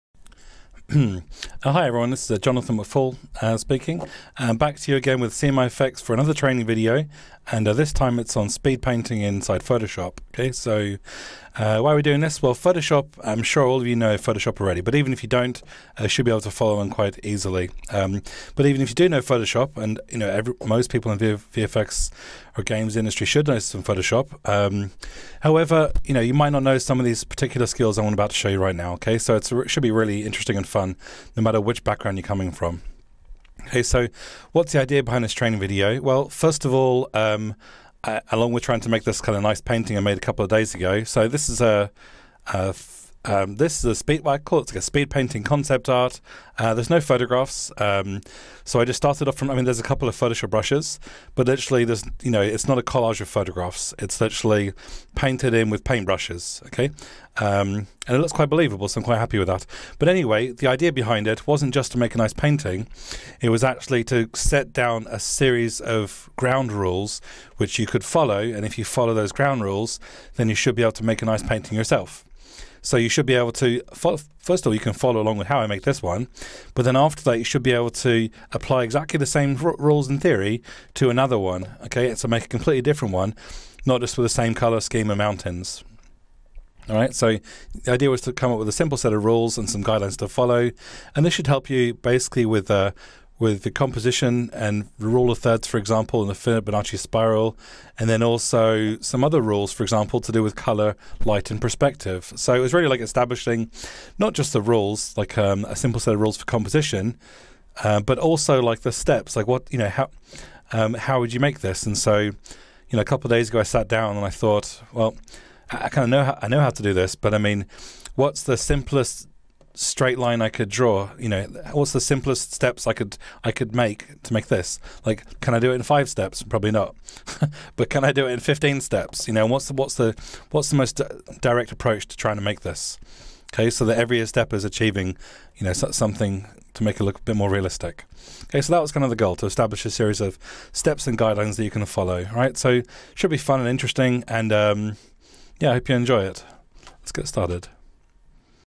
Photoshop Speed Painting For Concept Art And Environments 101
In addition to speed painting concepts and brushes, Jonathan teaches you concepts such as compositions, the rule of thirds, the Binoche spiral, and several rules to do with color, light, and perspective.